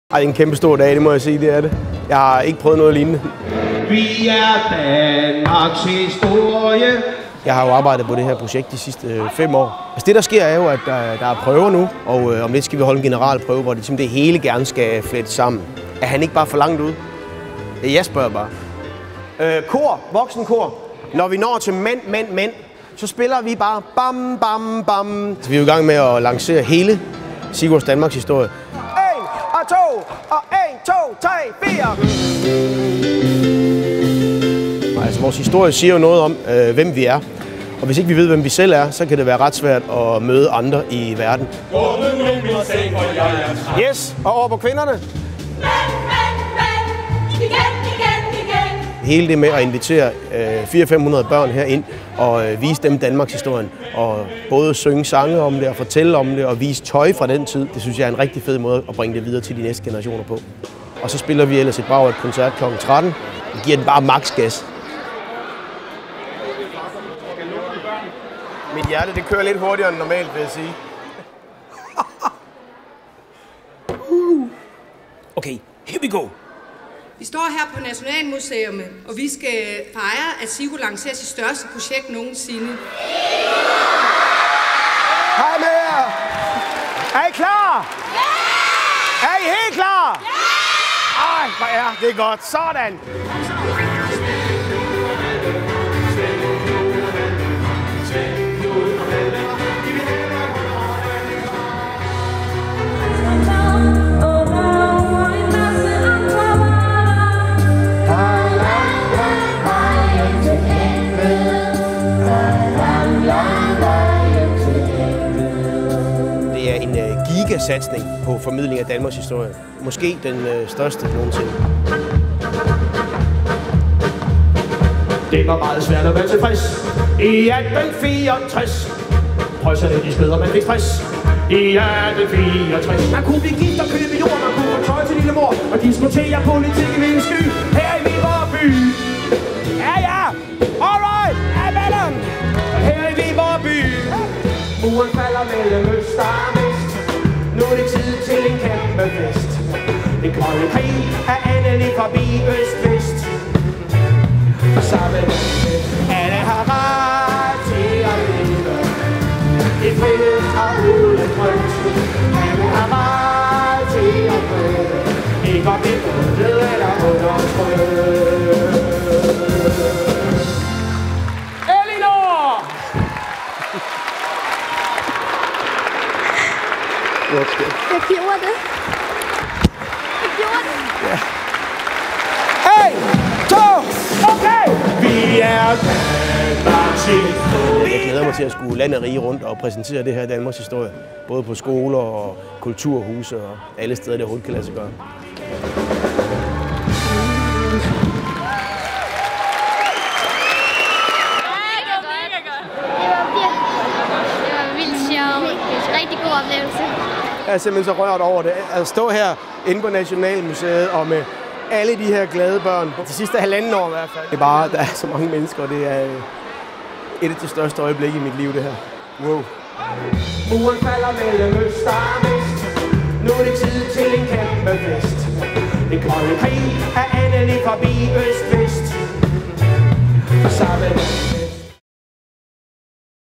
Sigurd fortæller Danmarkshistorie - Nationalmuseet okt 2016
Lanceringen af Sigurds Danmarkshistorie

Nationalmuseet dannede rammen for et af de største formidlingsprojekter for børn til dato; Sigurds Danmarkshistorie. 500 børn så på, da en af vort lands mest folkekære entertainere lancerede sit største og mest ambitiøse projekt til dato. Sigurds Danmarkshistorie blev opført live med stort orkester, dansere, og et 80 personer stort børne- og voksenkor.

I 2017 vises Sigurd fortæller Danmarkshistorie som tv-serie på DR Ramasjang, og i hele 2017 og 18 begiver Sigurd sig ud på en omfattende turné, der rummer koncerter på 100 skoler samt en lang række kulturhuse og museer.